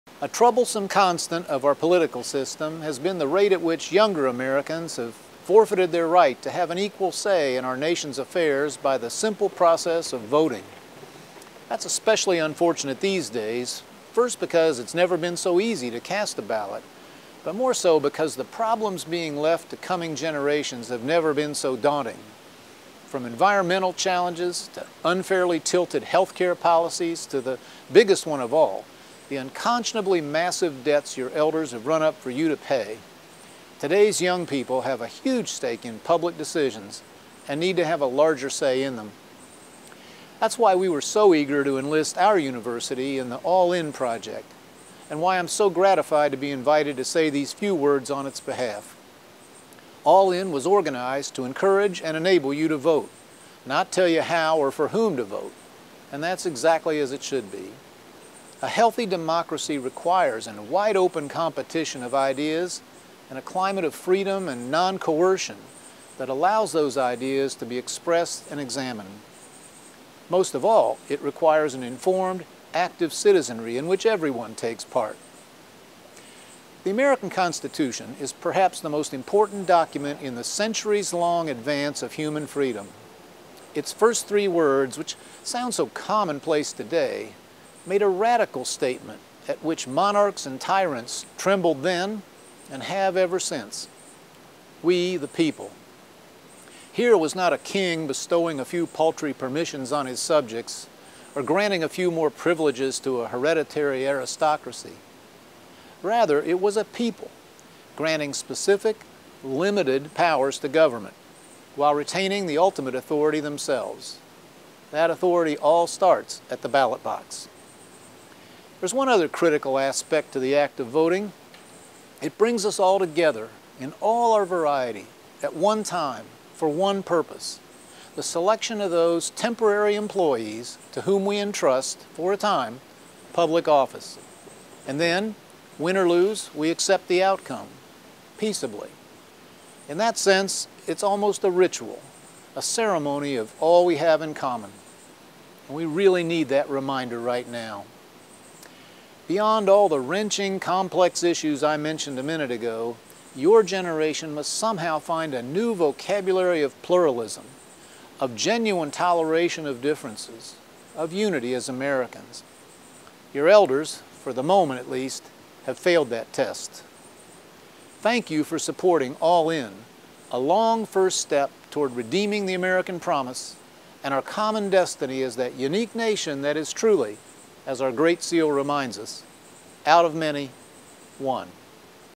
Purdue President Mitch Daniels Reflects on the Importance of Voting
In this video statement, Purdue President Daniels talked about our common destiny as Americans.

He expressed concern at the number of younger Americans who have forfeited their right to vote considering their generations’ stake in upcoming public decisions. He calls for younger voters to participate in the constitutional rights given to them by American citizenship.